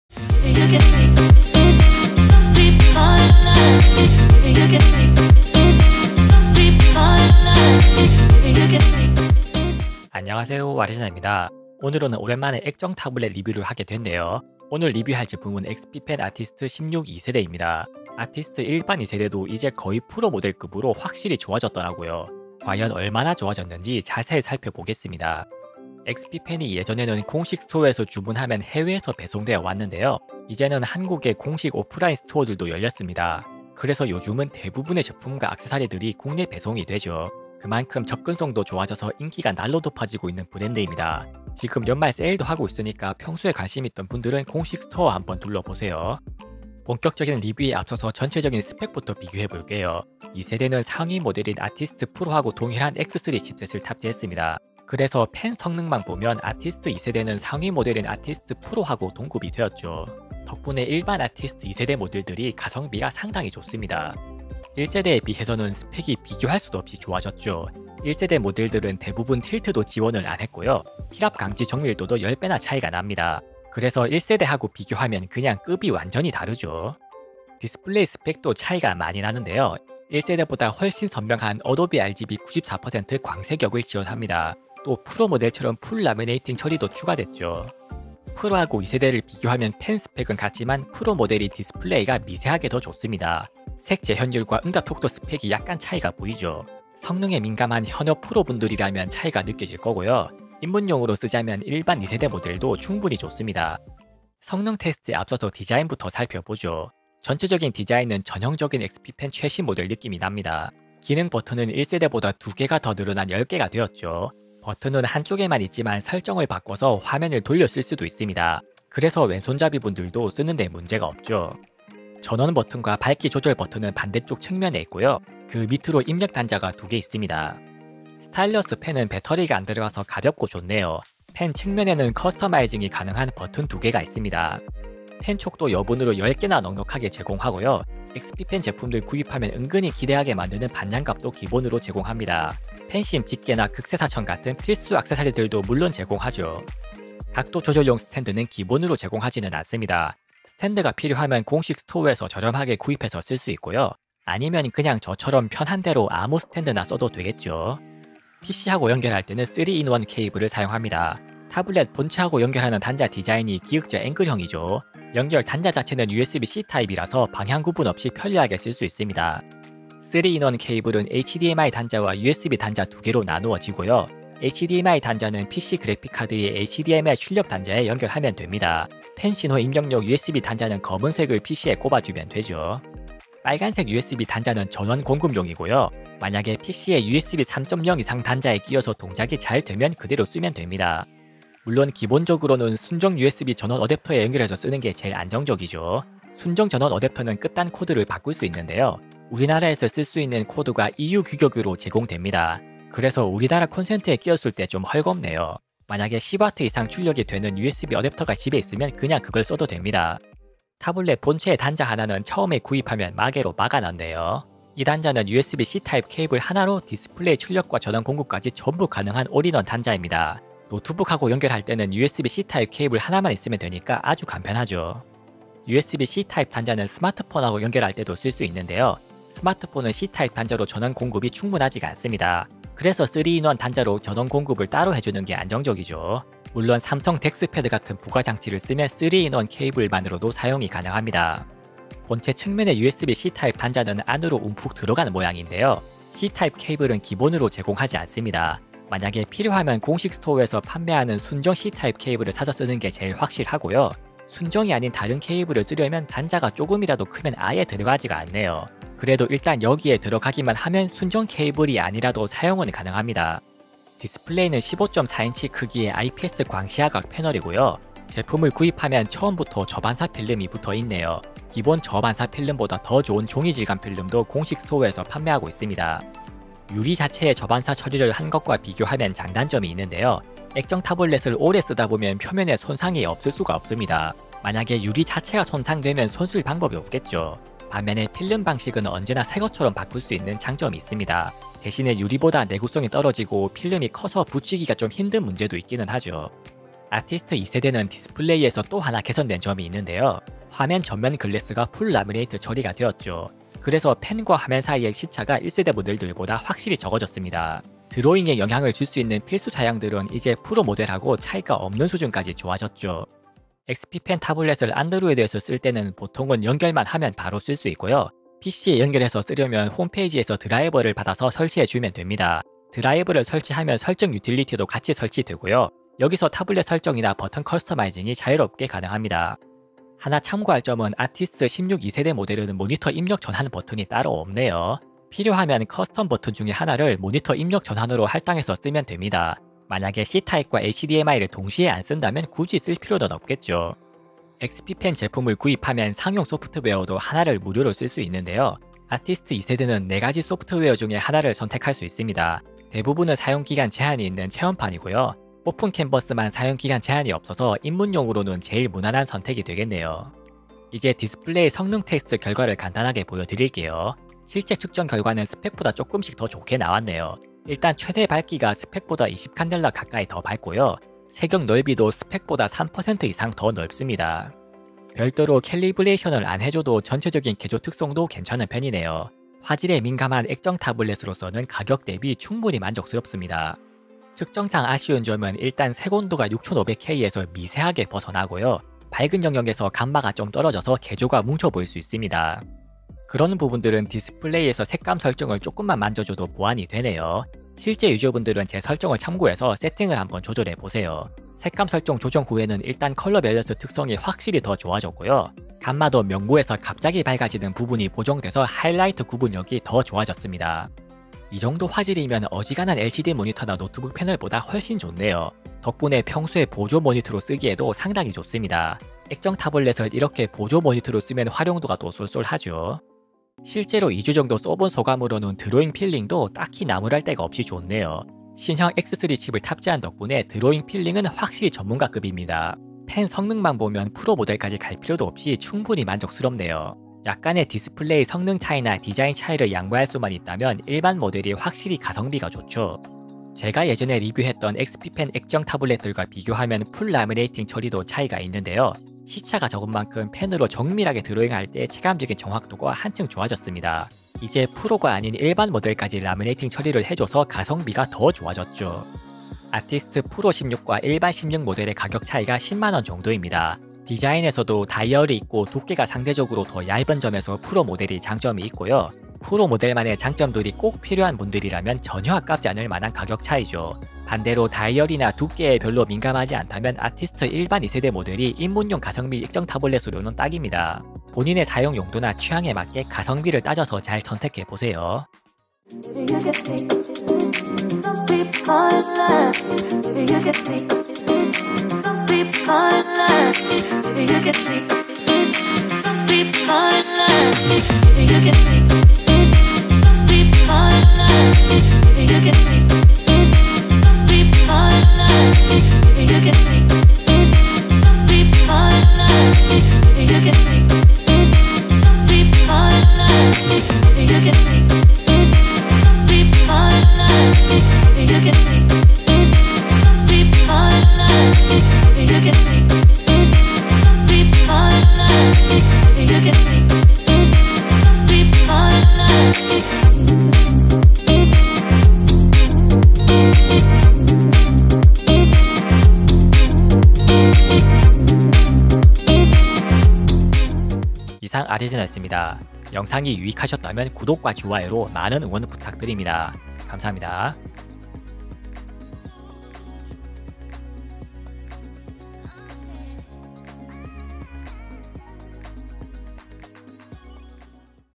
프로급 성능으로 가성비 향상! 👍 XP-PEN Artist 16 2세대 엑스피펜 액정 타블렛 리뷰
1세대와는 다르다! 1세대와는!

INDEX

0:00 미리보기
0:10 제품 개요
0:46 스펙 비교
1:44 디자인
2:38 연결 : HDMI 3-in-1
3:30 연결 : USB C타입
4:23 디스플레이
5:13 드라이버, 무료 제공 소프트웨어
6:00 디스플레이 측정
7:03 드로잉 필링
8:05 드로잉 10000배속
9:01 아웃트로

본 리뷰는 XP-PEN 코리아의 제품 및 비용 지원으로 진행되었습니다.

제조사나 구독자 분들께서 리뷰할 만한 제품을 보내주시면 순도 100% 솔직하게 평가해 드립니다 :)